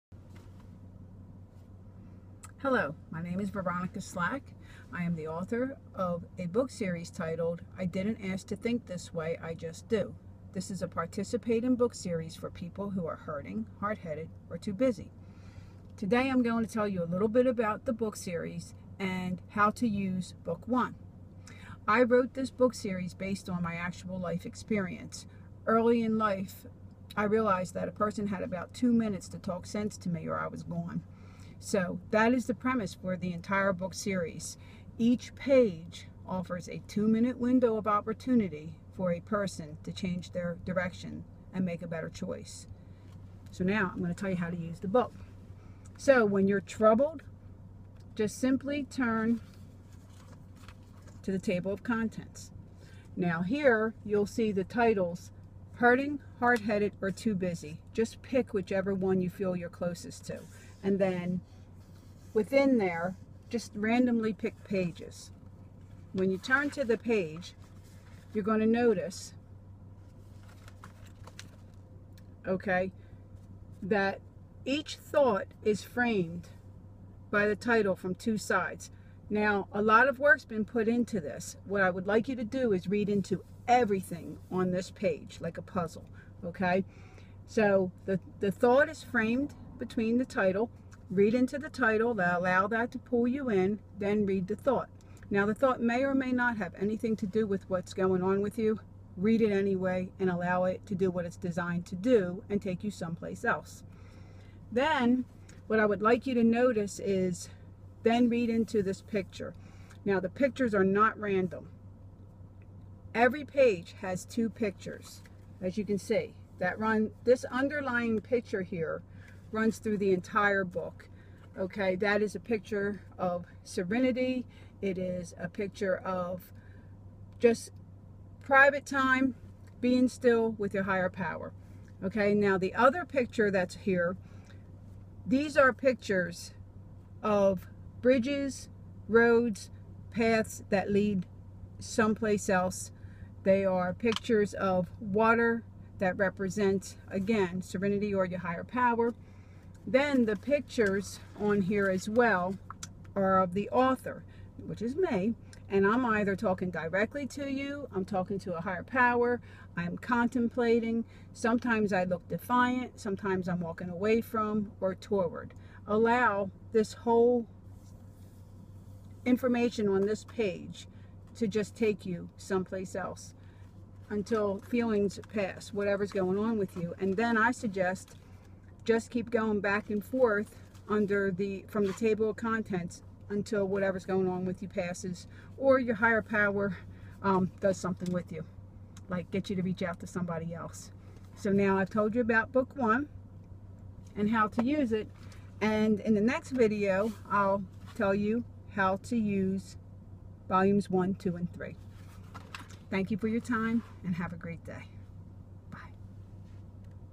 Veronica Slack -How to use Book I, when troubled
How to use Book I from her Book Series, I didn't ask to think this way, I just do...

The participating book series is designed to encompass all genders, all cultures, and all beliefs,
by way of the reader participating or co-writing the books of the series, unique to their own views.